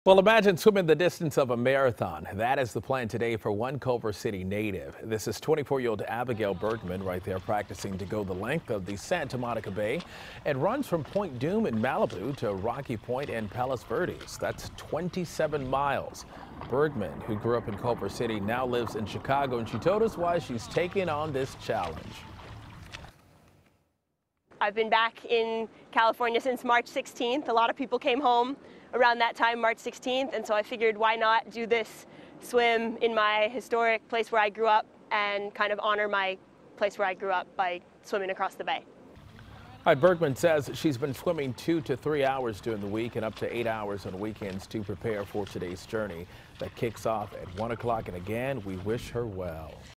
Culver City Native To Attempt 27-Mile Swim
Abigail Bergman, who had been living in Chicago, came back to Southern California in March. DeMarco Morgan reports.